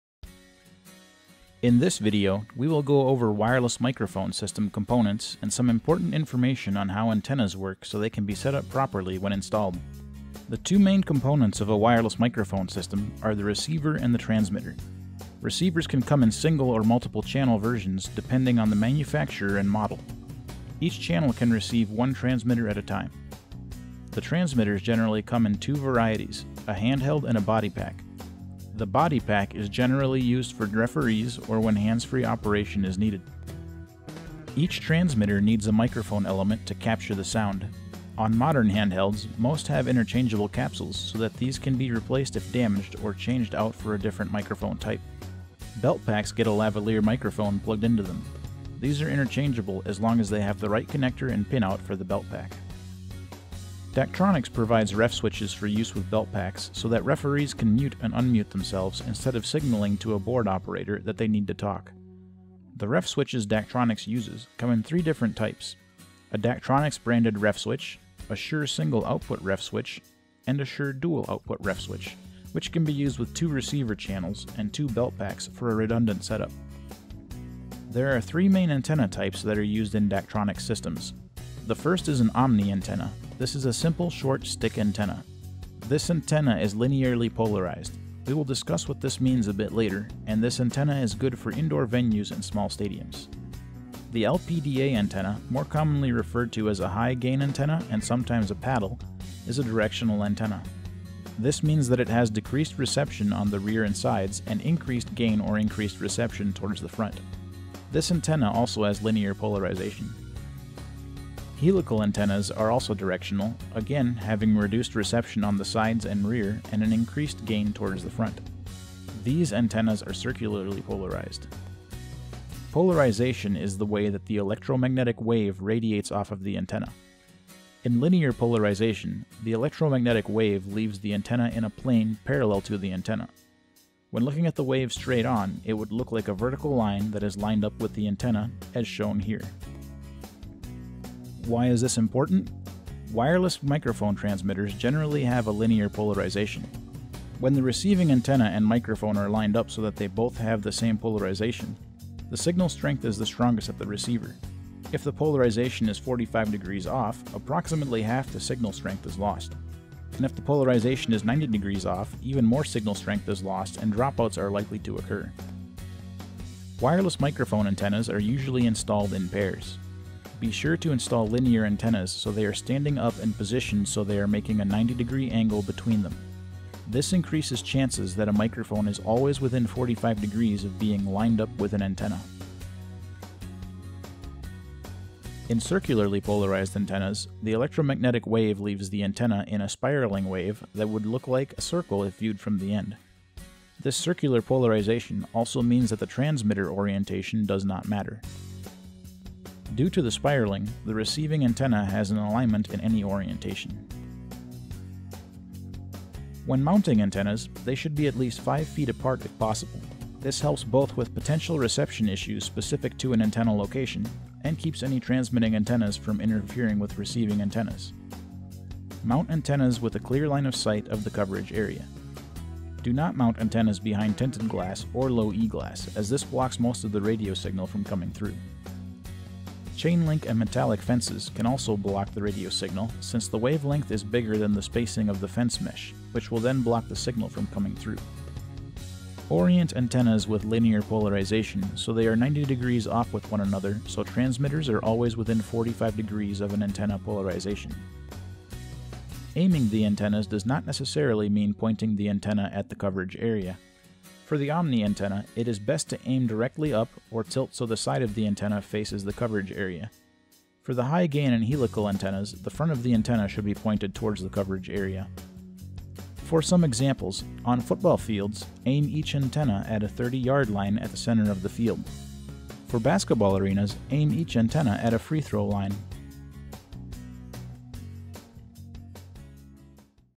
Wireless Microphones: Components and Antennas
This video provides an overview of wireless microphone components and antennas used in Daktronics Audio Systems.